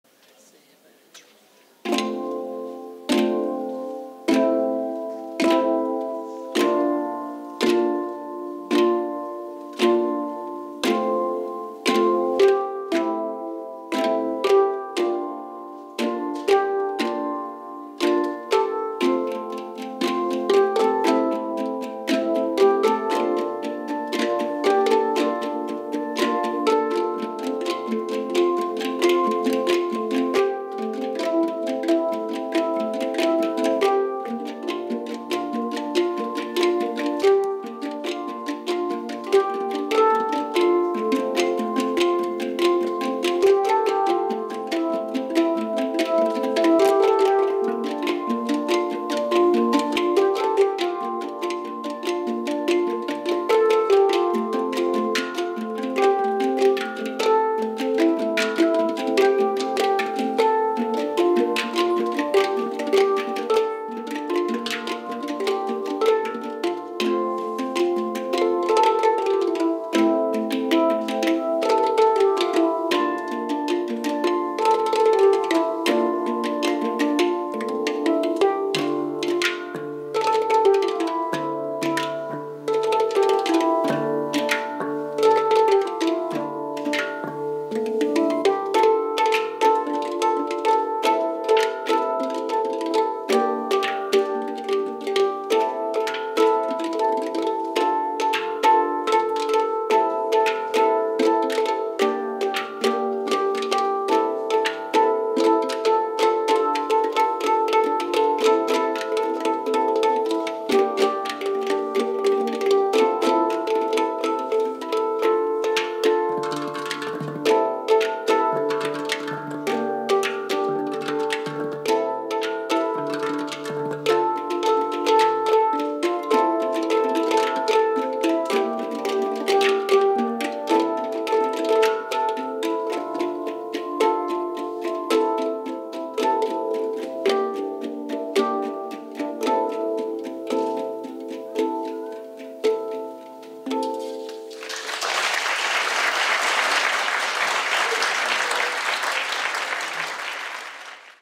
Hang Drum ensemble Zavieh Music Institute Concert Arasbaran Concert Hall Tehran winter 2023
Juveniles who have trained year